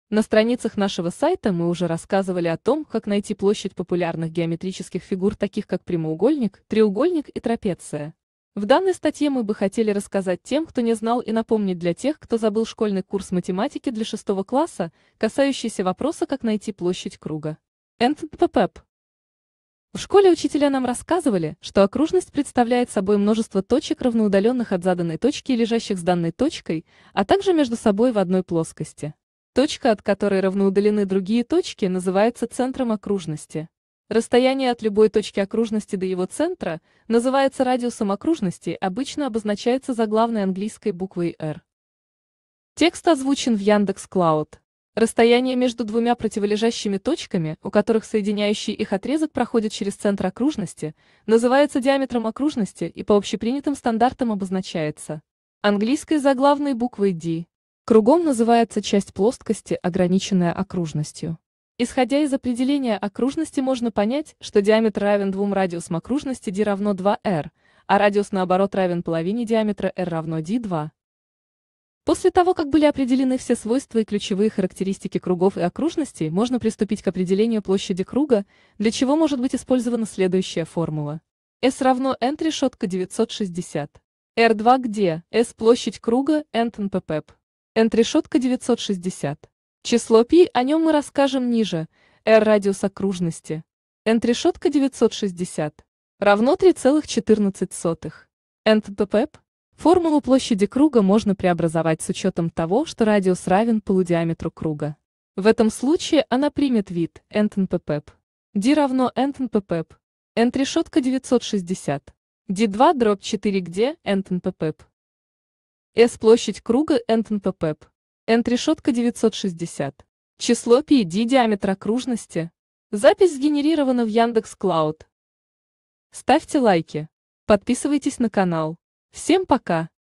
Как найти площадь круга?
Понятие круга и окружности как геометрических фигур. Нахождение площади круга по формулам через радиус или диаметр окружности. Величина числа пи, используемого для нахождения площади.